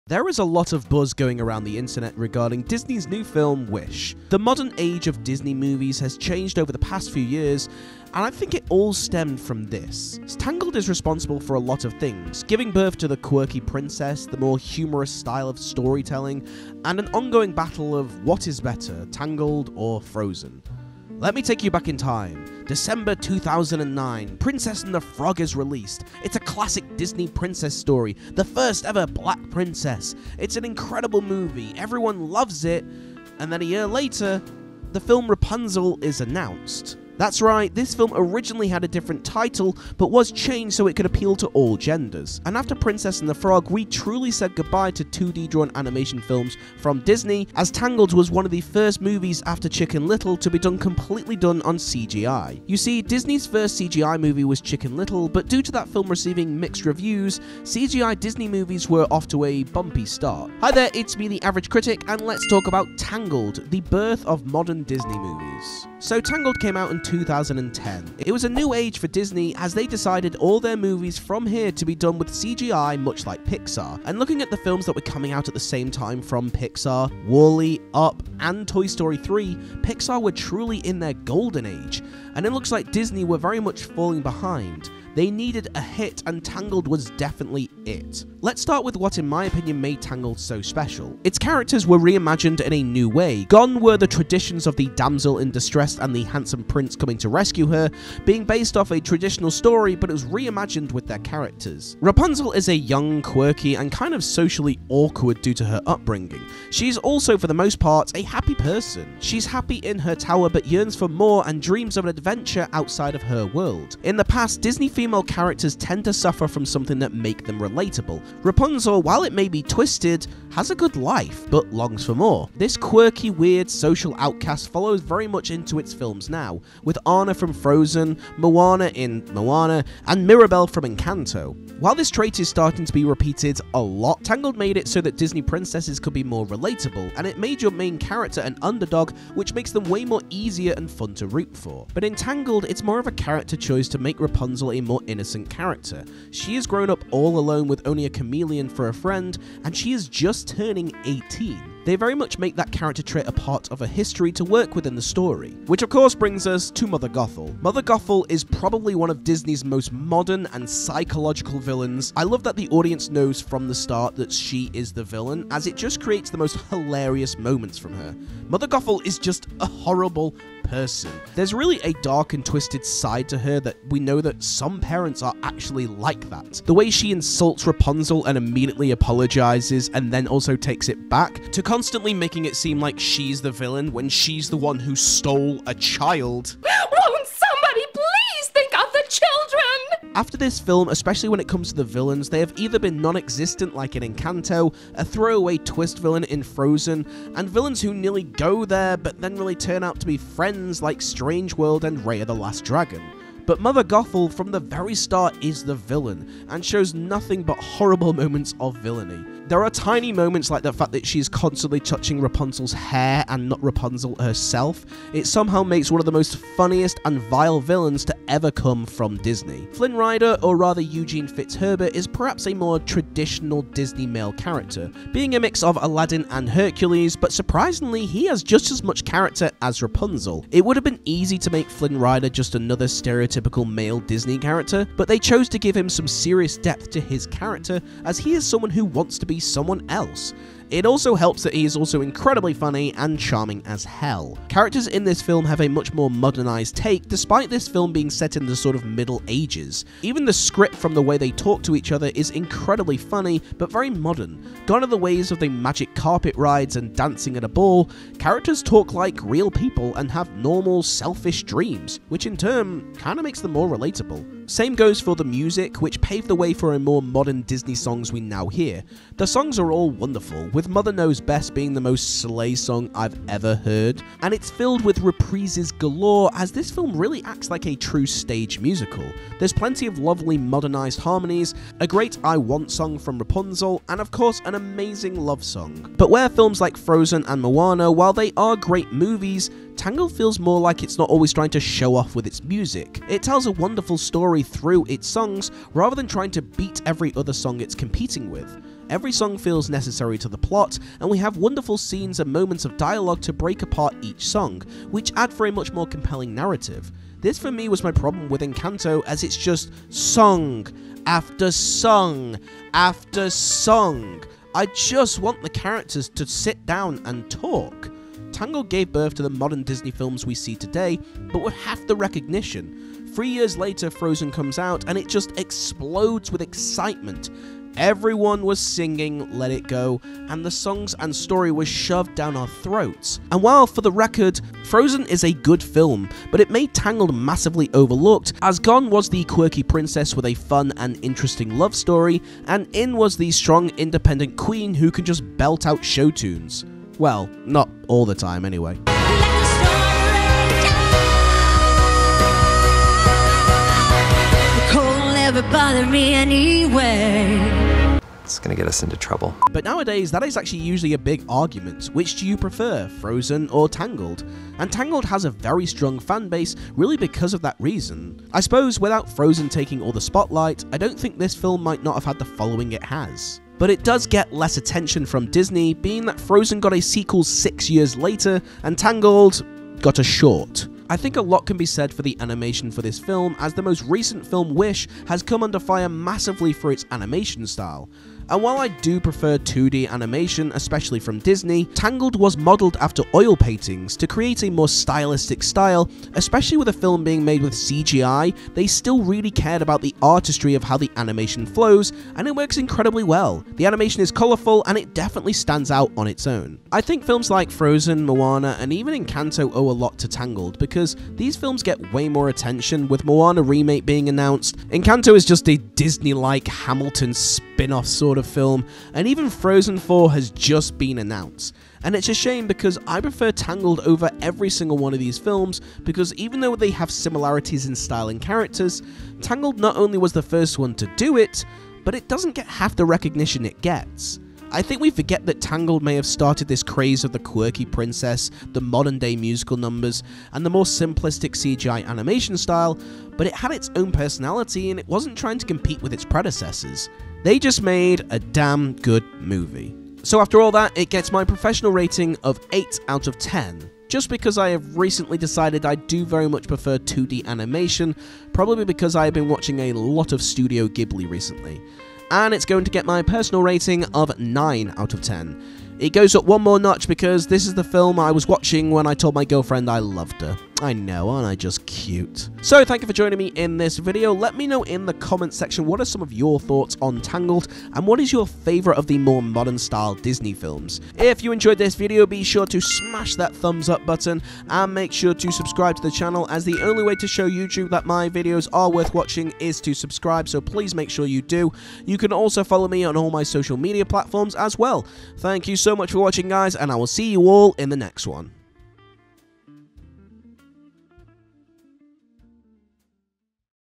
Tangled: the birth of modern Disney films
So before Wish comes out lets talk about the film that started it all. Enjoy!